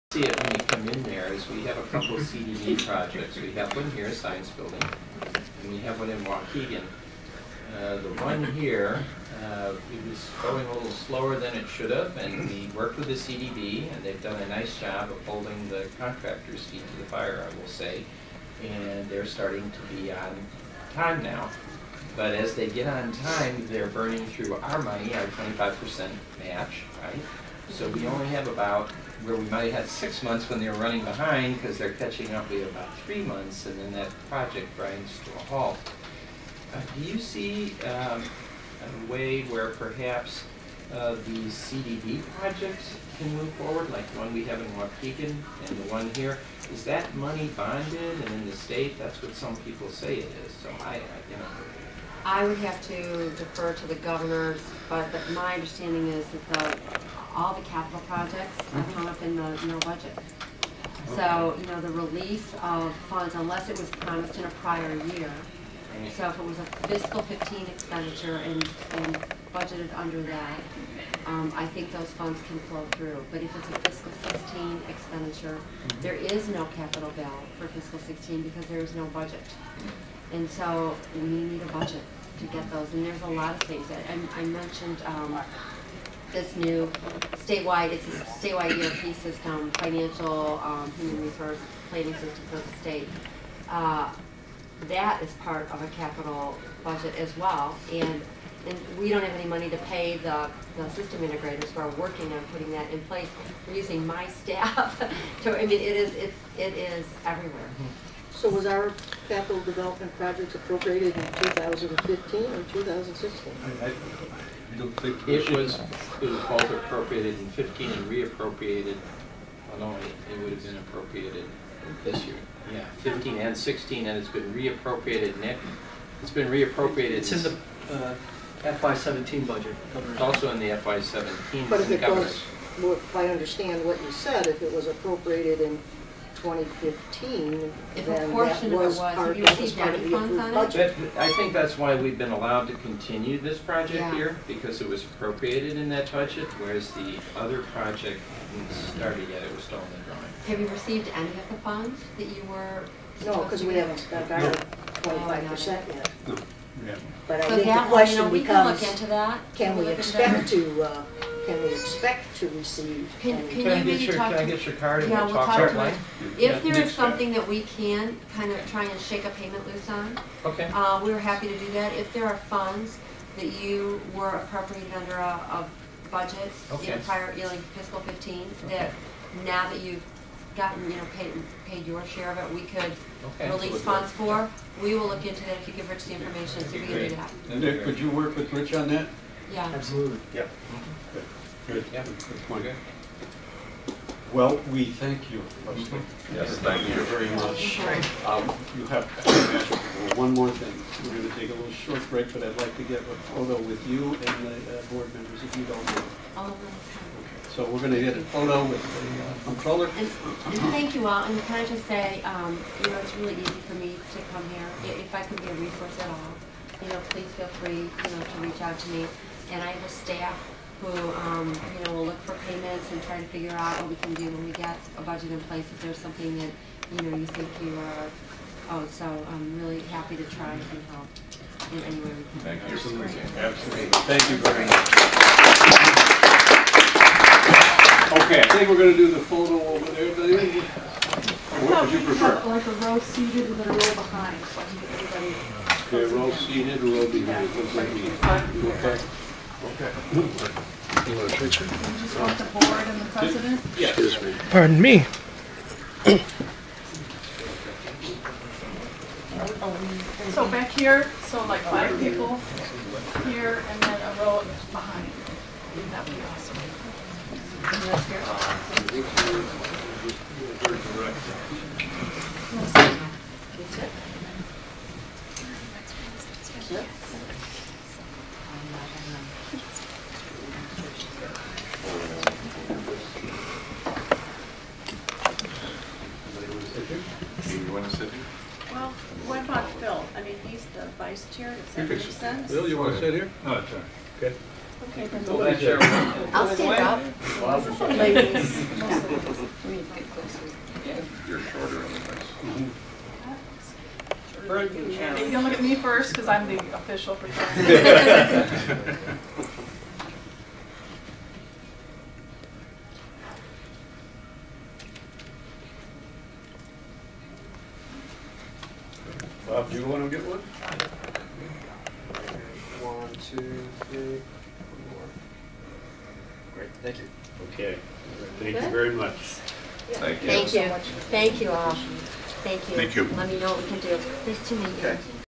CLC Board - February 2016 - Comptroller Reports to the Board 4 of 4
Leslie Geissler Munger, Illinois State Comptroller reports on the state of the Illinois budget deficit and how that affects CLC and other colleges in Illinois.